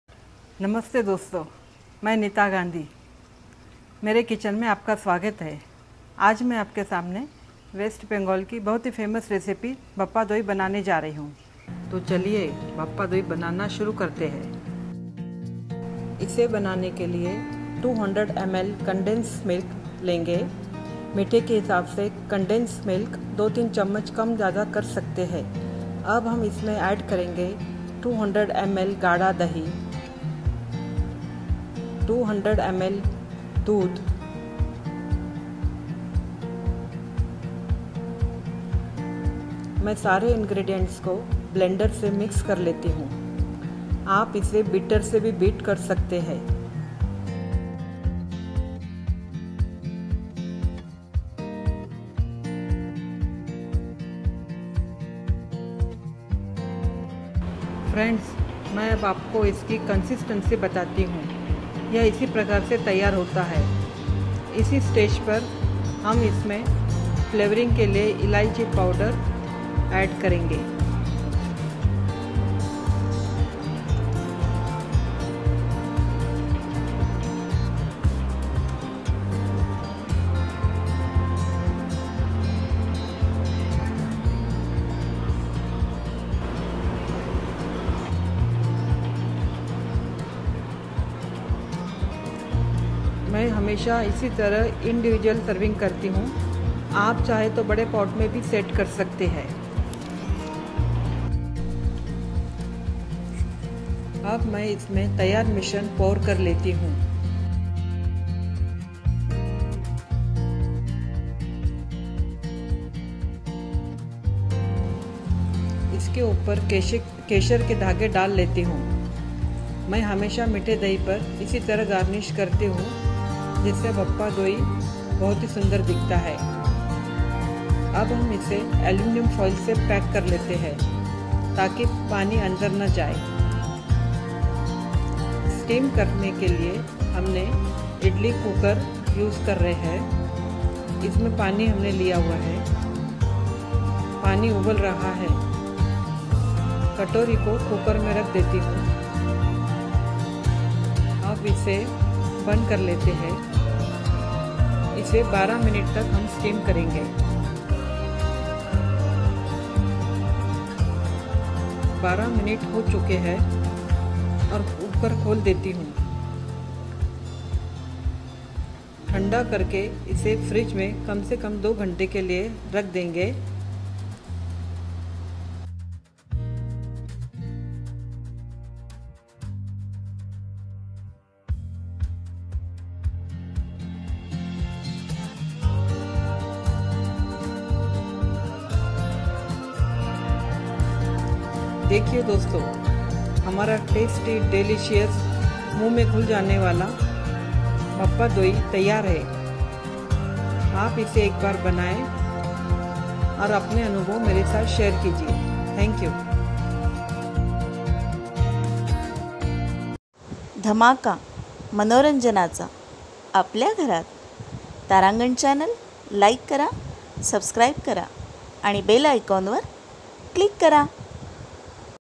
Neeta Gandhi presenting Bhappa Doi (Sweet Curd) dish for Tarangan Channel| नीता गांधी | तारांगण
अकोल्याच्या नीता गांधी ह्यांनी सादर केलीय प. बंगालमधील प्रसिद्ध भप्पा दोई (गोड दही) ही स्पेशल डिश.